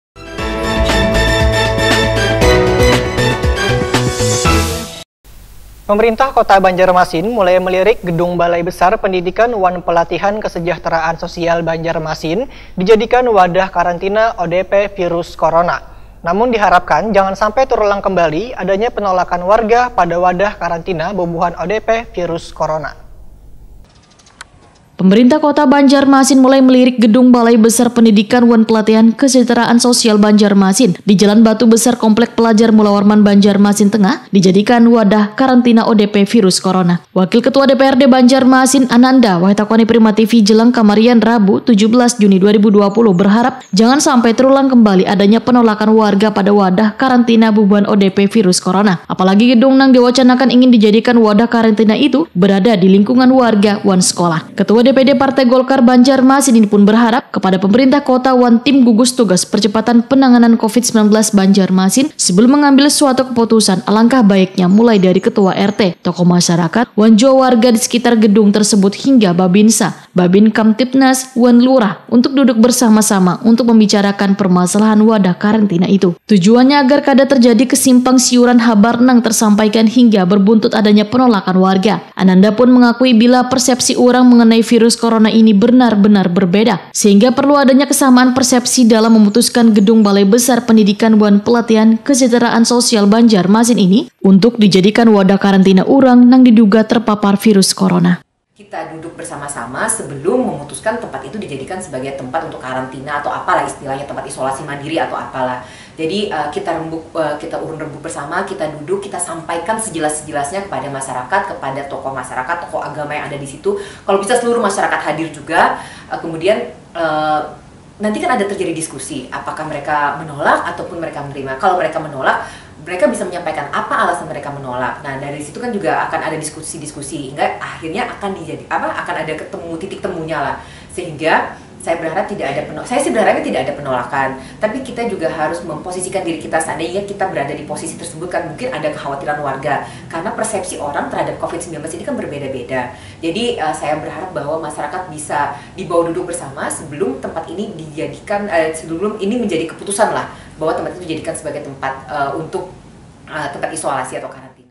DEWAN MINTA PEMKO BANJARMASIN DUDUK BERSAMA WARGA UNTUK MENENTUKAN WADAH KARANTINA VIRUS CORONA
Pemerintah Kota Banjarmasin mulai melirik gedung Balai Besar Pendidikan wan pelatihan Kesejahteraan Sosial Banjarmasin di Jalan Batu Besar Komplek Pelajar  Mulawarman Banjarmasin Tengah/ di jadikan wadah karantina ODP Virus Corona//  Wakil Ketua DPRD Banjarmasin – Ananda wayah ditakuni prima tv/ jelang kamarian Rabu 17 Juni 2020 berharap jangan sampai terulang kembali adanya penolakan warga pada wadah karantina bubuhan ODP Virus Corona//  Apalagi gedung nang diwacanakan ingin dijadikan wadah karantina itu berada di lingkungan warga wan sekolah//